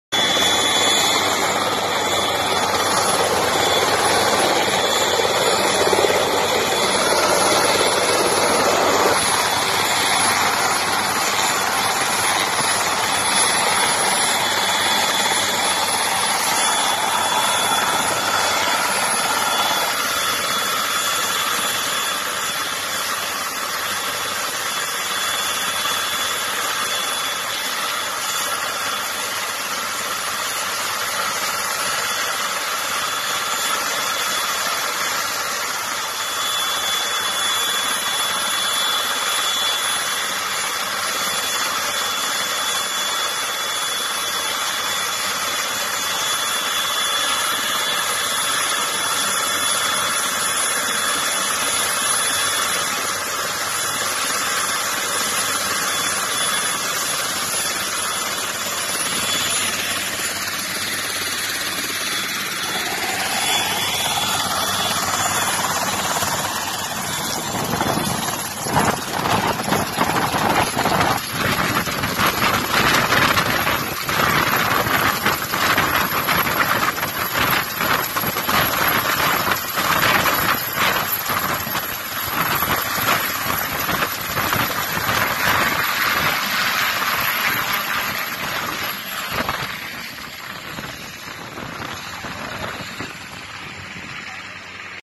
भारतीय वायु सेना के 114 एचयू लेह ने एक साहसी बचाव अभियान में माउंट नून बेस कैंप सेएक घायलपर्वतारोही
भारतीय वायु सेना के 114 एचयू लेह ने एक साहसी बचाव अभियान में माउंट नून बेस कैंप से एक घायल पर्वतारोही को सफलतापूर्वक निकाला @jammutawi1756